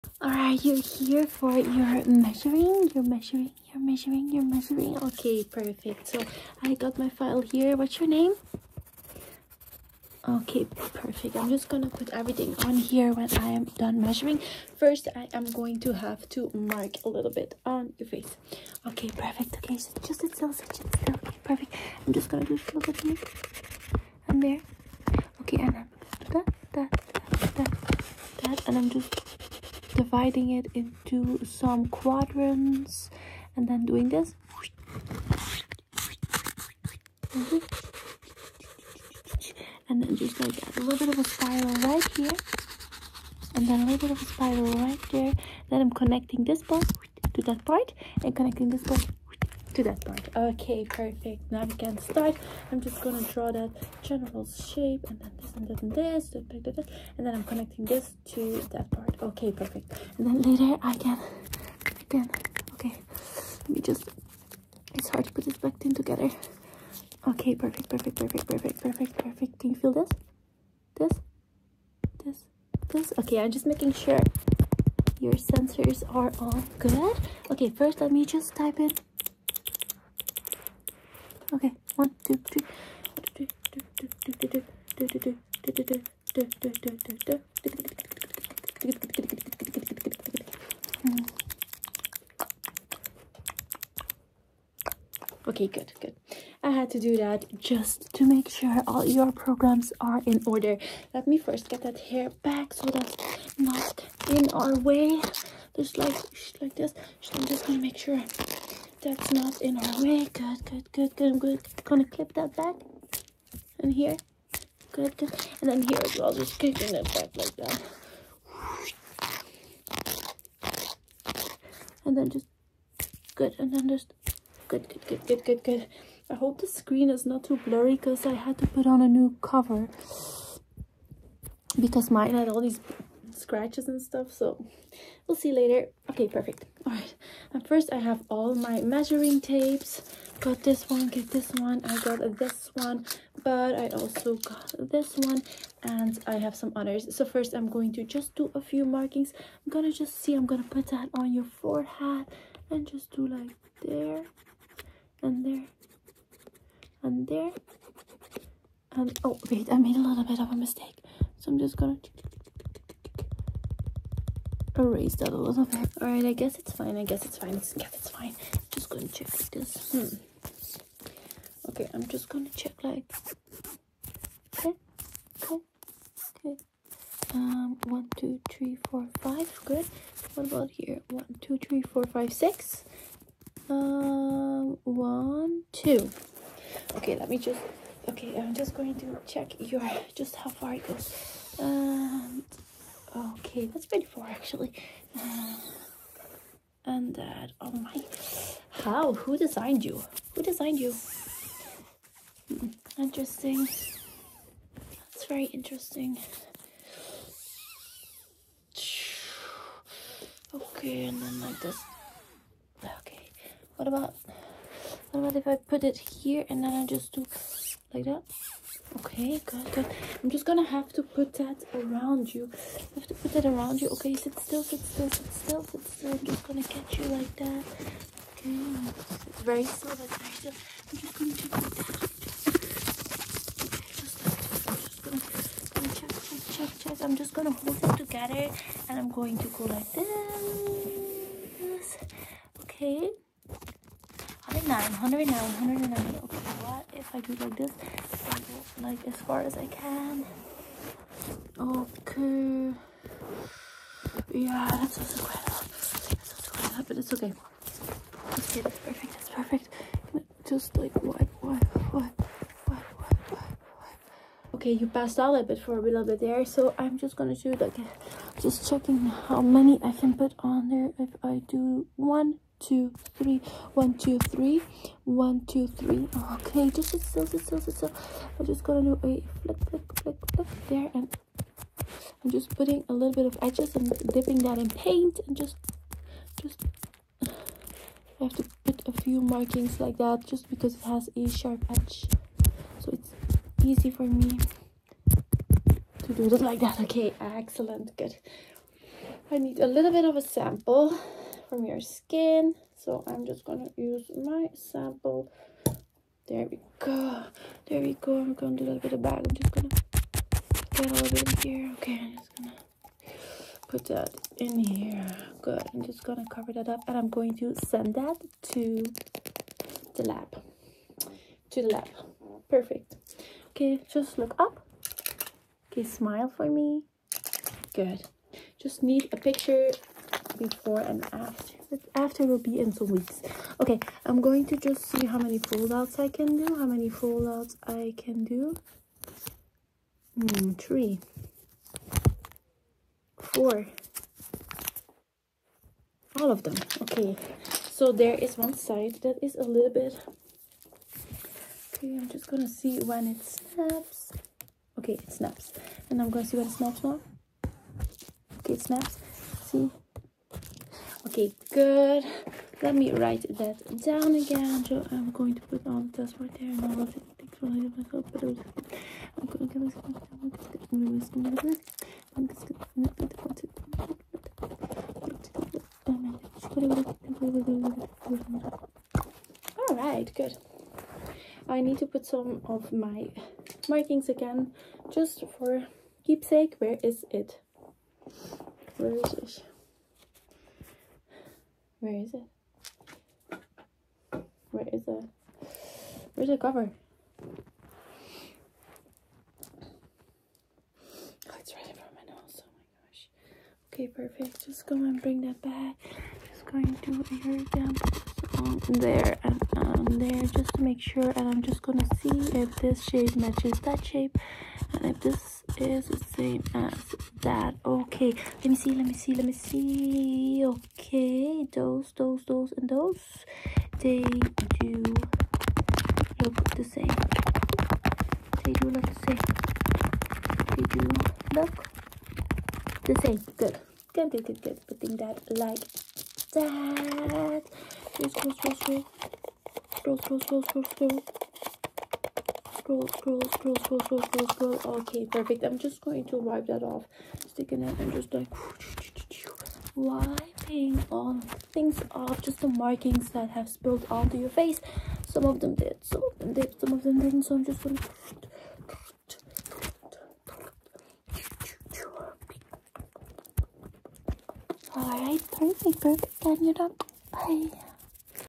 FAST, CHAOTIC & Aggressively Face Measuring ASMR ⚡📏(Lofi)
In this video I am chaoticly fast and aggressively measuring you with all kinds of tools, scribbling, soft speaking, face touching etc... perfect asmr for tingles, people who are bored, adhd, sleep and tingles.

BTC: 15QkA2eU3SCM27P5S4jFADcAcmiCYmmMf2 ETH ERC20: 0x8e4d233ed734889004ad878ab30f0e219646a81c LTC: LgKwn4kJQWCAWZ73yecb8VqrrLAVrG8JbF DOGE: DQb1o5eh9MShr5HE6856F7HaEPTMCkuS4p

(exclusive monthly video(s) for members!
► Bitcoin: 1JeRcYHSoJJxFGNLinw9BdrEAzQxKFBviF